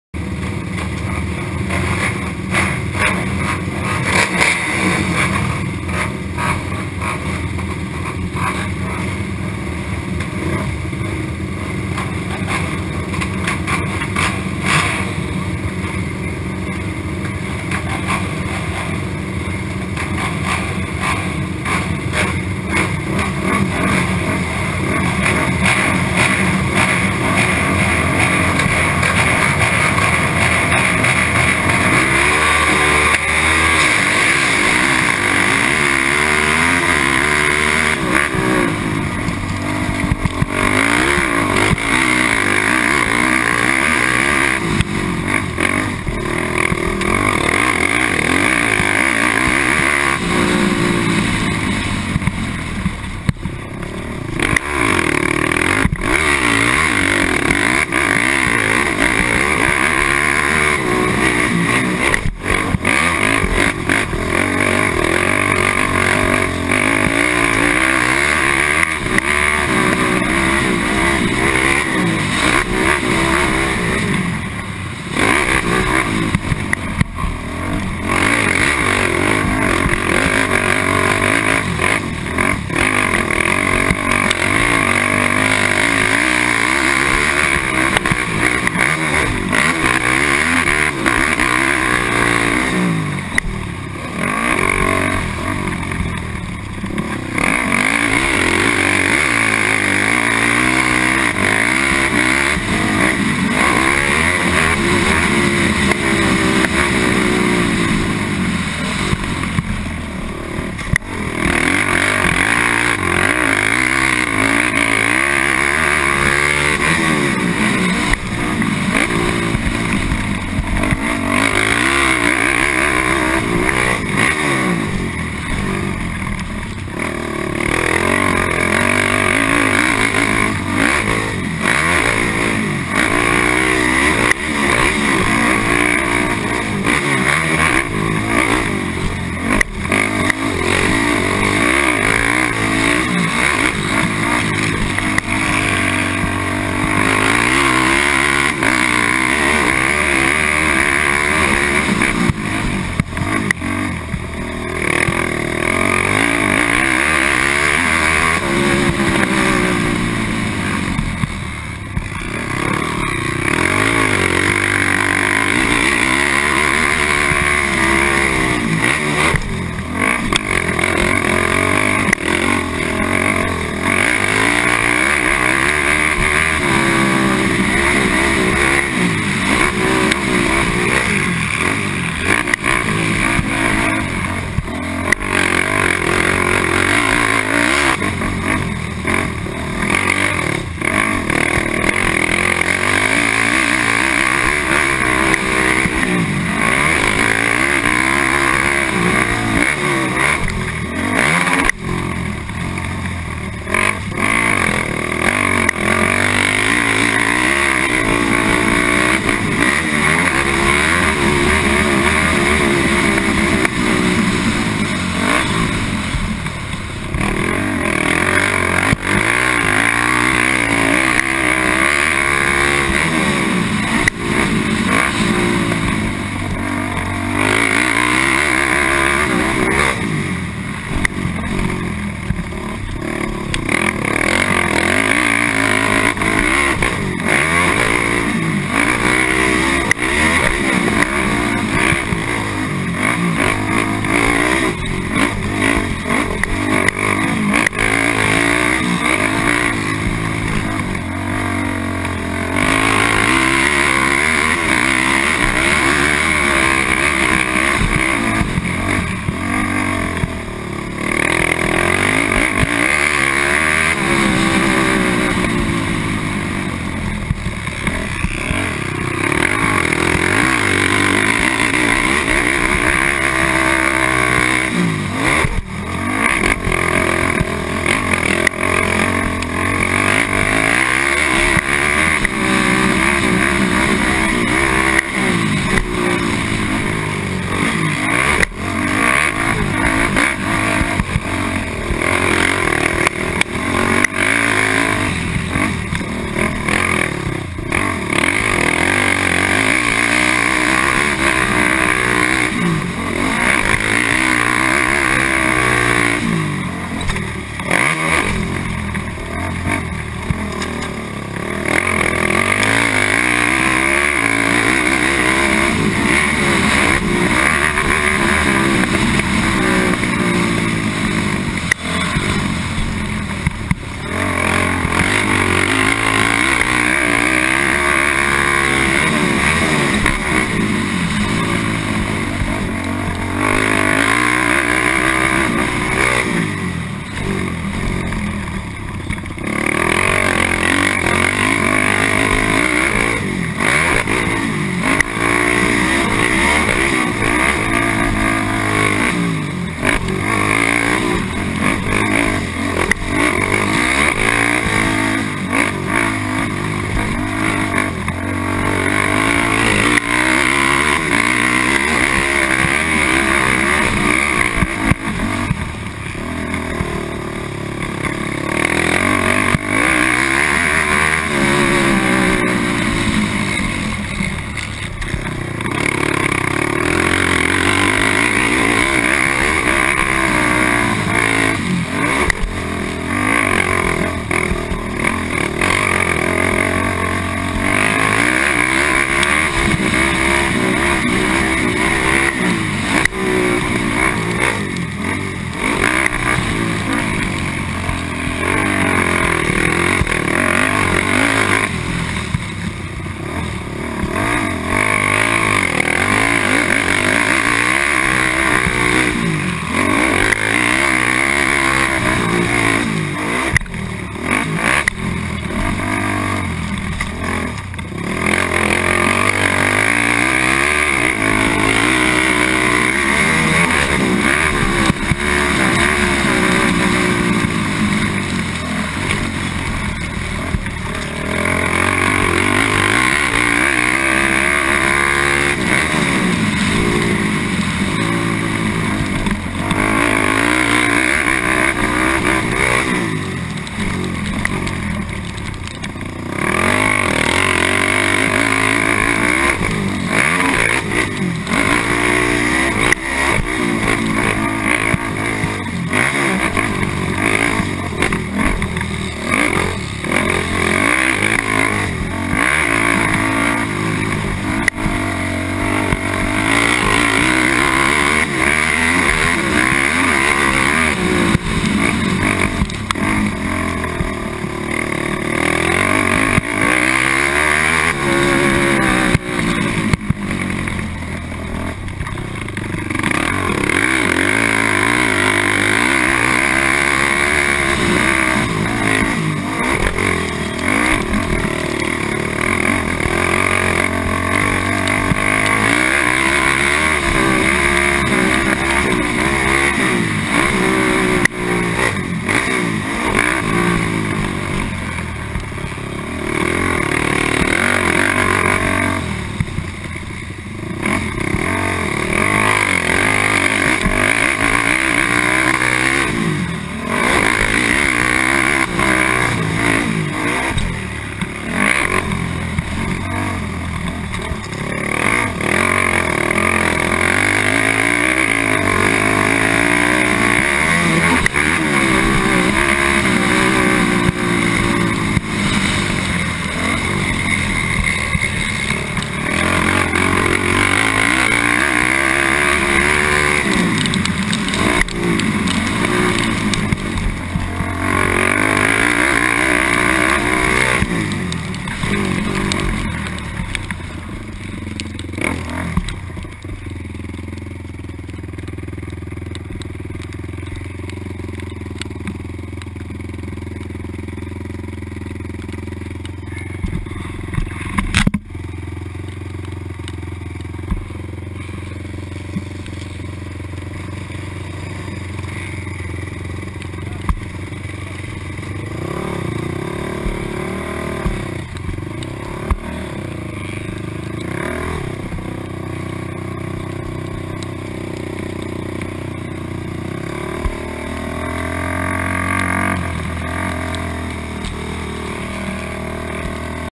Broome Tioga moto 2
Quad Open B with a A rider at the same gate drop